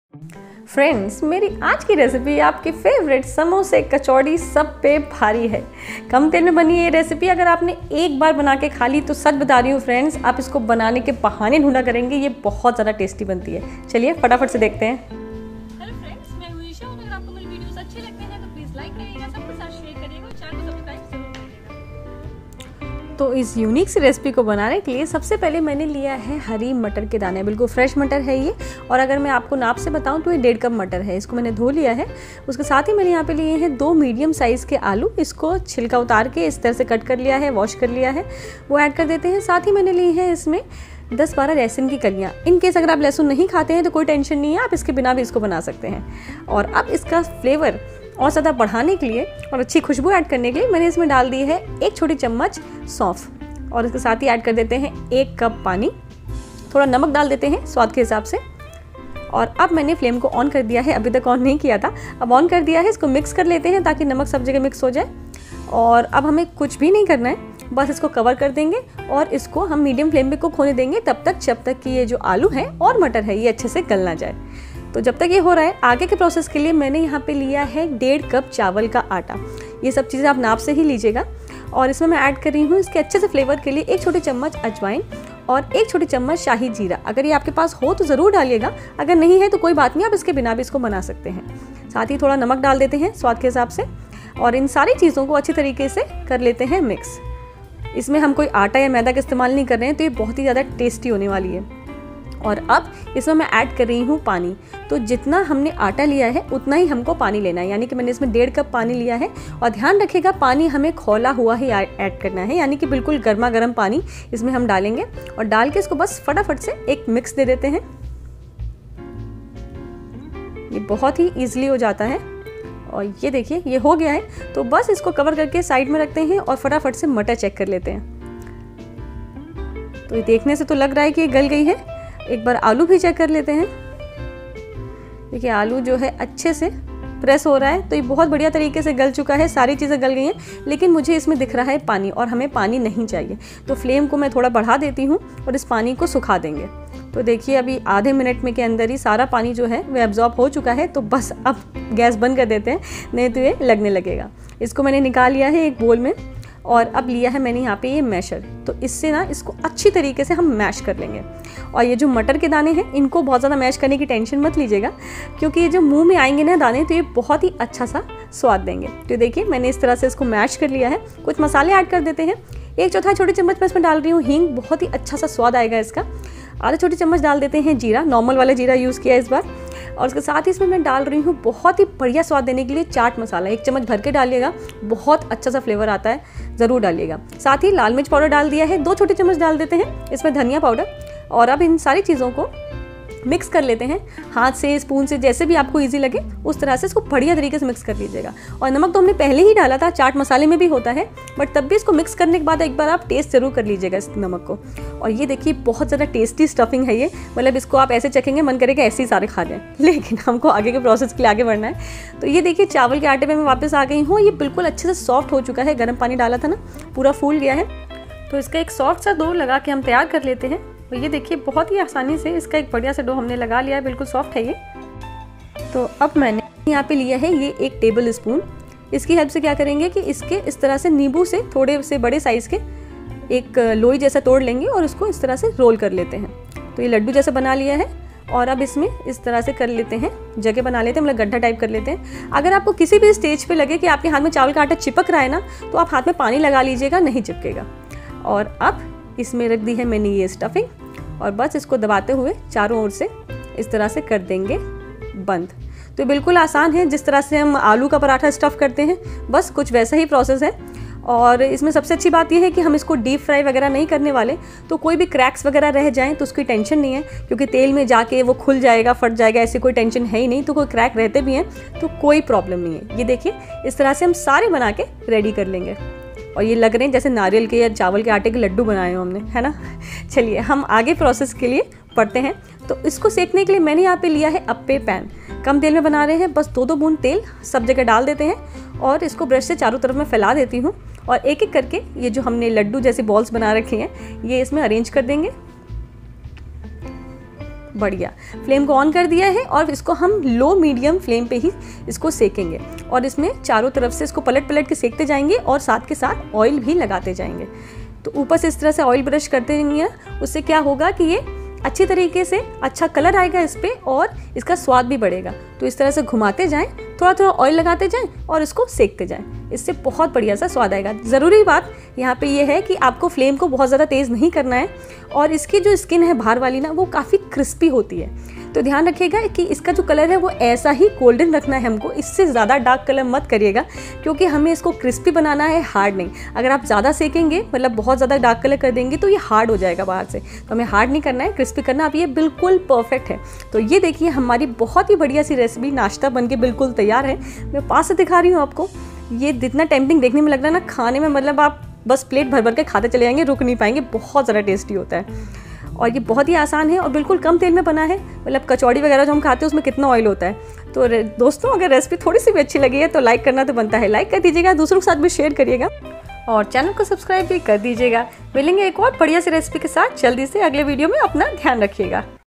कचौड़ी समोसा सब भूल जाओगे अगर एक बार मेरी ये रेसिपी बना के खाओगे | less oil recipe
हरी मटर से कम तेल में बना ये नाश्ता पूरे परिवार का दिल जीत लेगा | must try

Portable External HDD - USB 3.2 for PC Laptop Windows and Mac